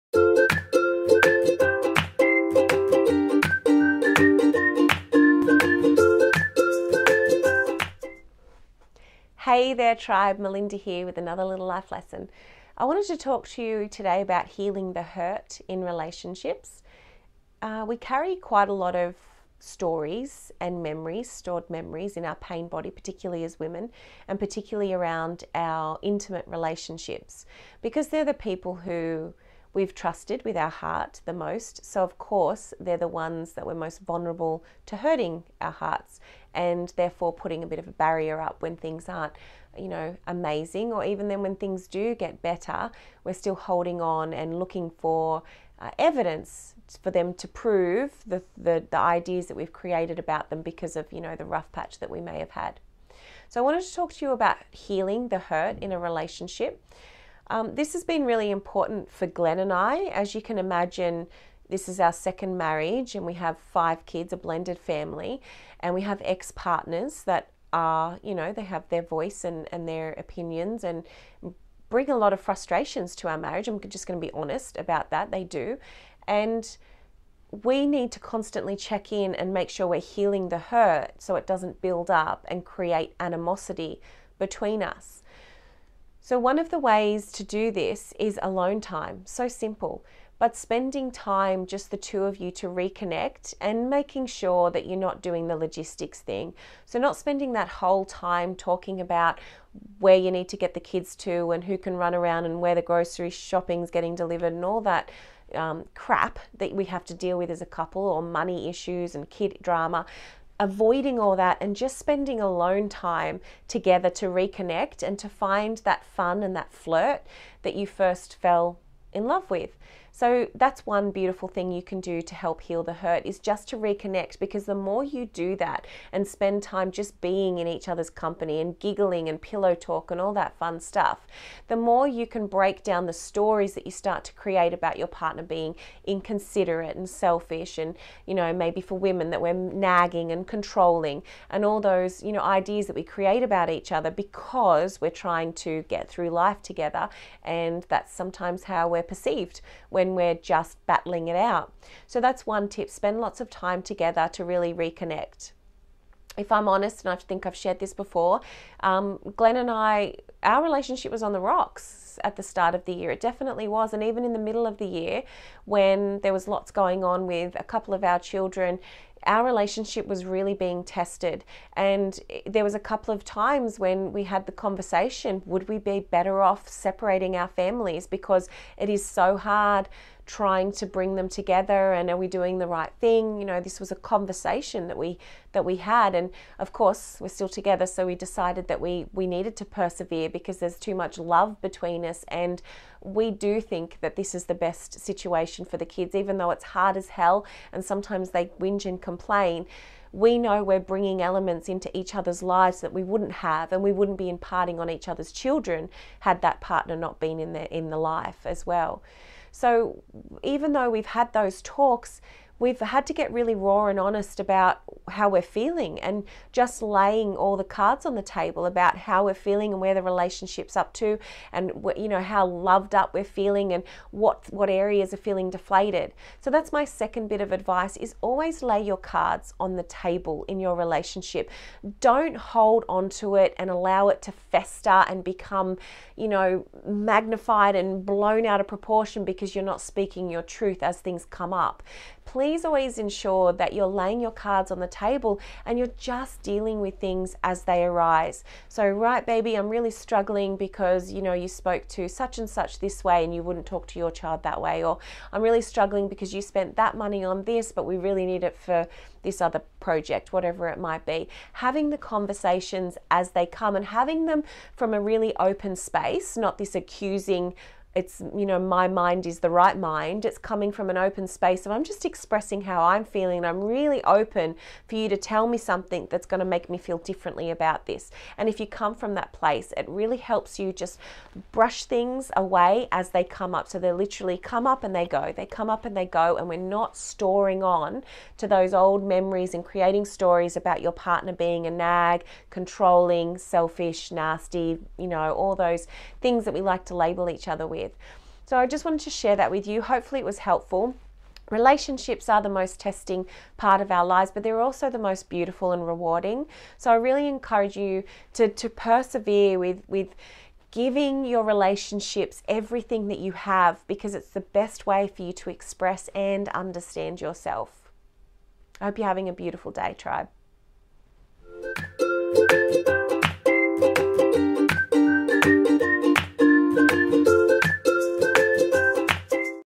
l i t t l e ~ l i f e ~ l e s s o n
Glen and I have worked hard this last 6 months to keep our relationship alive.
It has been challenged with our family drama and needed extra TLC to bring back the love and avoid the build up of resentment and frustration that occurs when you let stories get in the way of seeing your partner for who they truly are.
Sharing some concepts that help us. xx